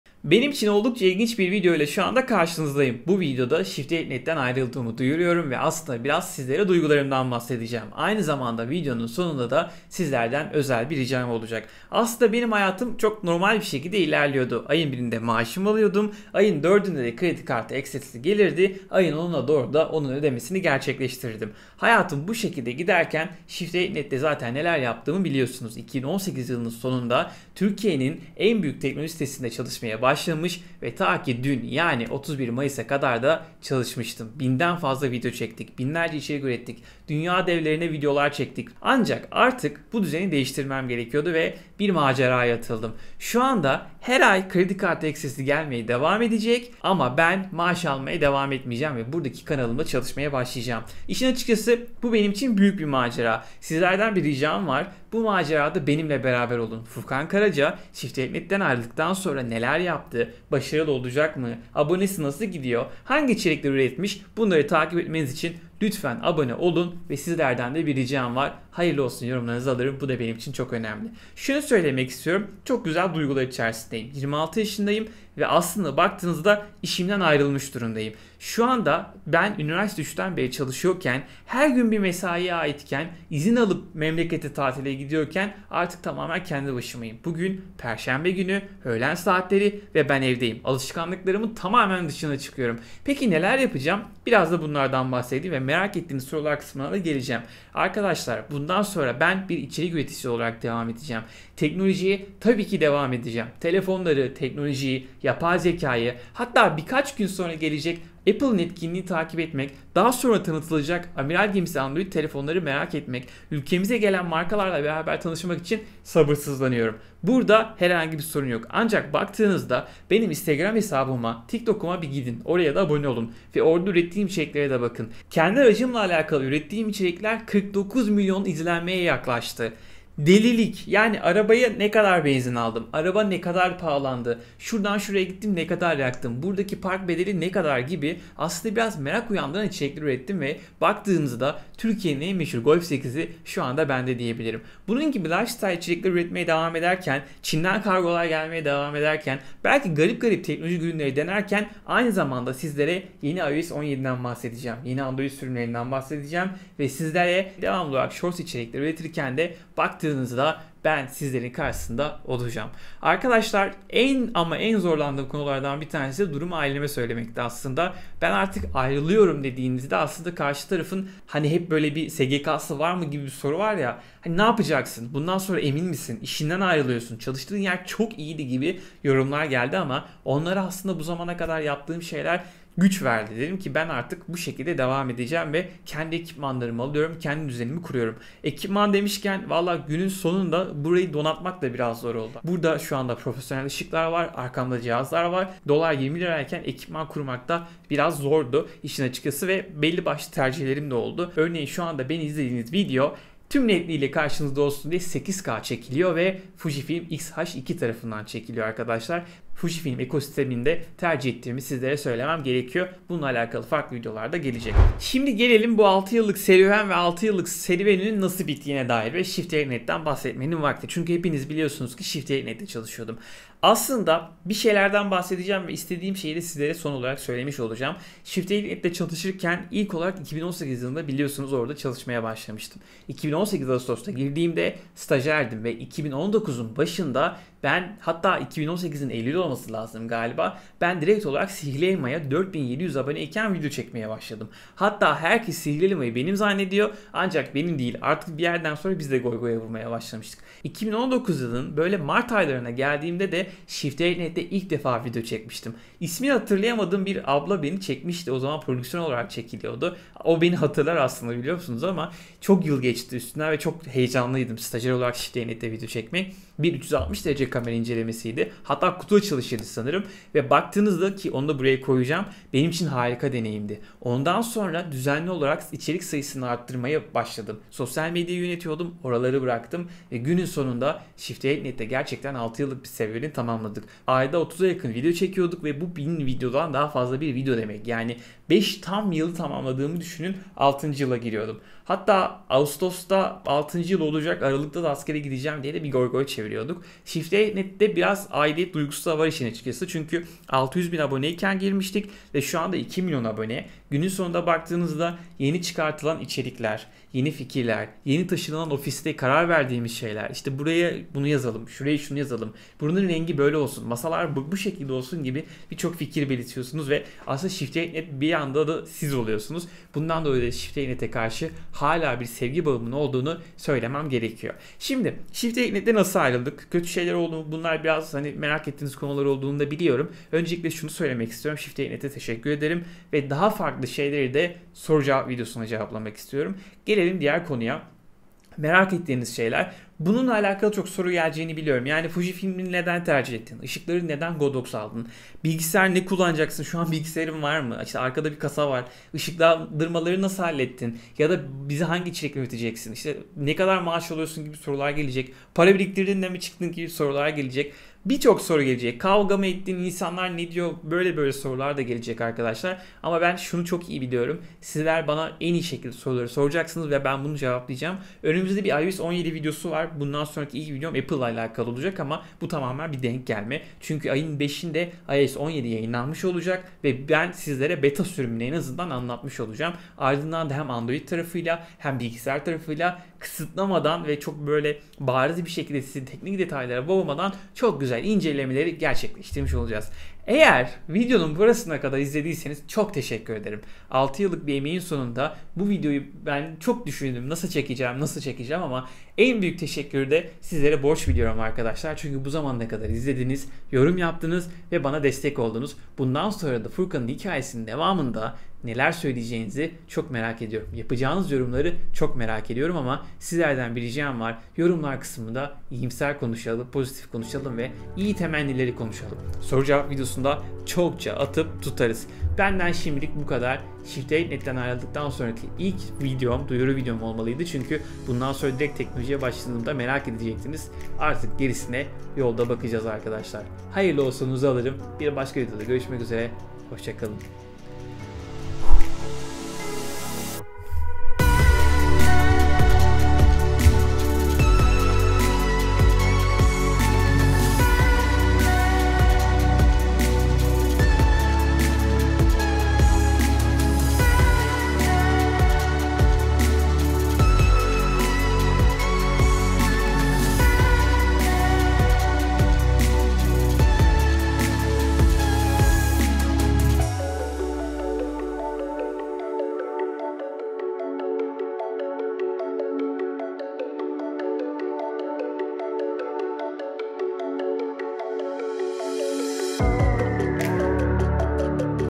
ShiftDelete.Net'ten ayrıldım!
Ben Furkan Karaca,

2018 yılından beri çalıştığım SDN'den Mayıs sonu itibari ile ayrıldım.
Ve bu ayrılık sonrasında sizlere hem duyurmak hem de belki merak ettiğiniz soruları gidermiş olurum diye düşündüm.

Bu video içerisinde marka barındırdığı için

Aklınızda eğer Furkan karaca sdn'den neden ayrıldı, furkan karaca shiftdelete neden ayrıldı gibi sorular varsa, yorumlar kısmına yazın diğer videoda konuşalım. Furkan ayrıldı ama neden ayrıldı cevapları da gelecek.

Bu zamana kadar bana çok şey katan ShiftDelete.Net'e ve Hakkı Alkan'a özel teşekkürlerimle..

[00:00:00] ShiftDelete.Net’ten Ayrılma Duyurusu
[00:00:13] ShiftDelete.Net’teki Kariyer ve Deneyimler
[00:01:18] Kendi İşine Başlama Kararı
[00:02:59] Yeni Ekipman ve Çalışma Düzeni
[00:03:39] ShiftDelete.Net’teki İlk Günler ve Gelişim
[00:05:00] ShiftDelete.Net ile Bağlar ve Teşekkürler
[00:06:01] Gelecek Planları ve İçerik Üretimi